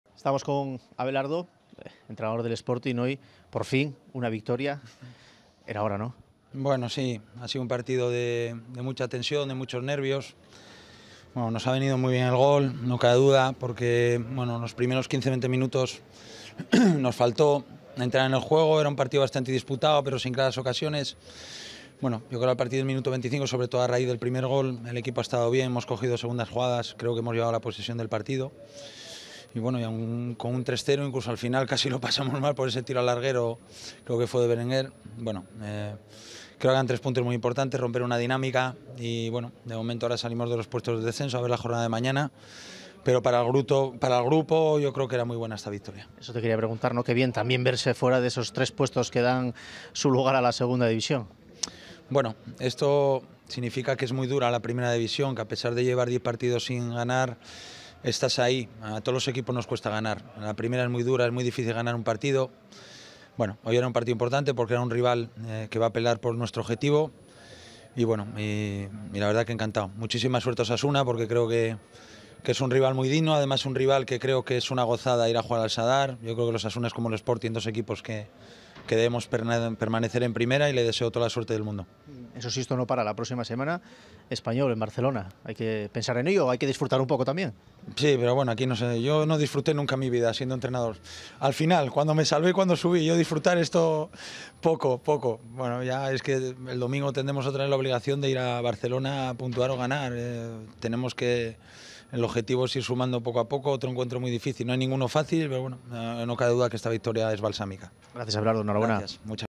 Abelardo: "Después del primer gol el equipo ha estado muy bien"
Entrevista a Abelardo tras el Sporting de Gijón vs Osasuna (3-1)